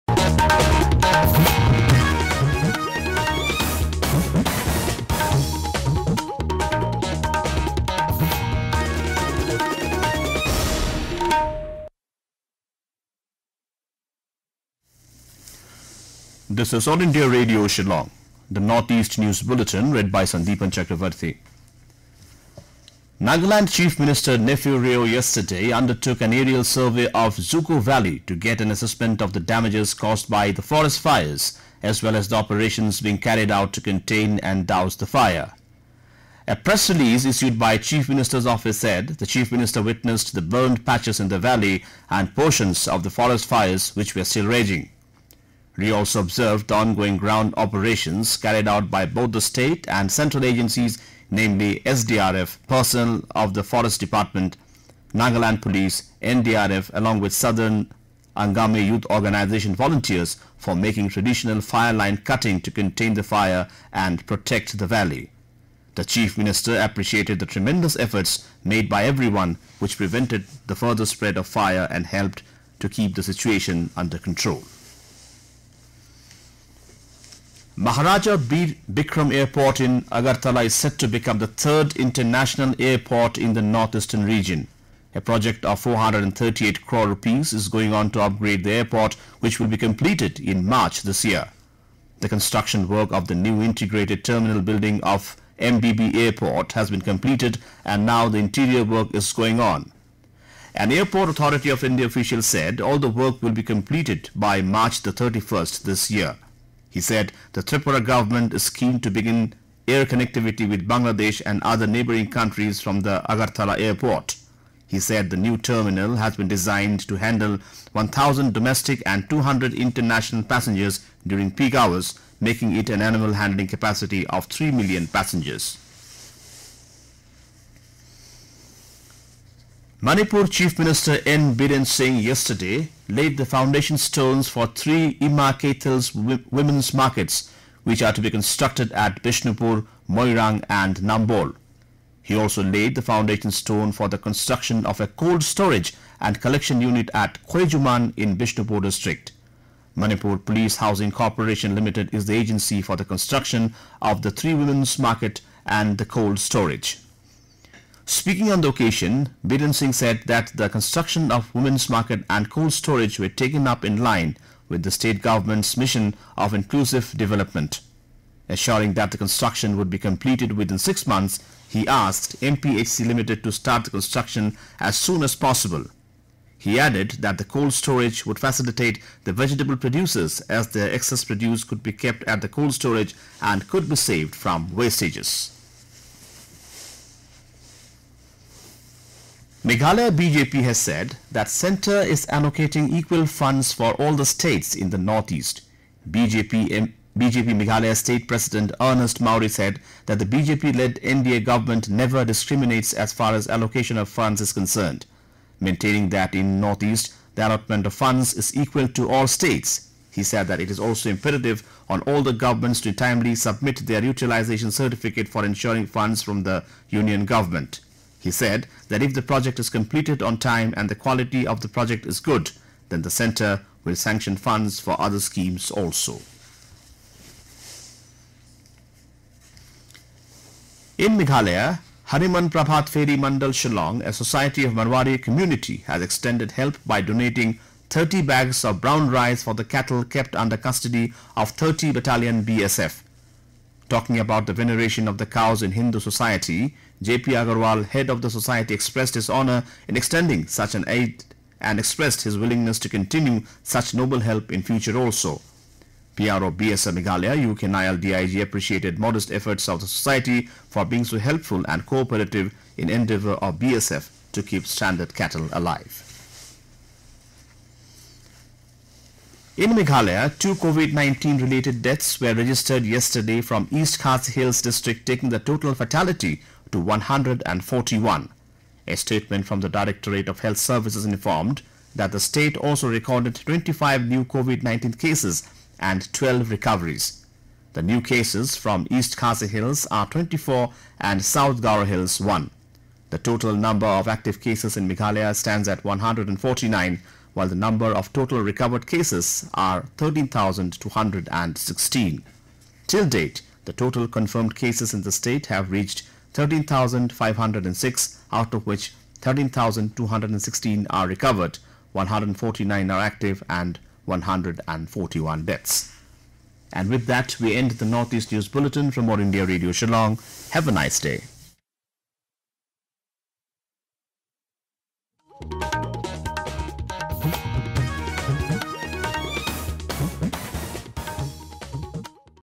NORTH EAST NEWS BULLETIN ON 06 01 2021
HEADLINES:

•  Nagaland Chief Minister Neiphiu Rio undertakes an aerial survey of Dzukou Valley to get an assessment of the damages caused by the forest fires.
• Maharaja Bir Bikram Airport in Agartala is set to become the third international airport in the Northeastern Region.
• Meghalaya BJP says Centre is allocating equal funds for all the States in the North East.
• Manipur Chief Minister N. Biren Singh lays the foundation stones for three Ima Keithels Women’s Markets.
• In Meghalaya, two COVID 19 related deaths were registered from East Khasi Hills district taking the total fatality to 141.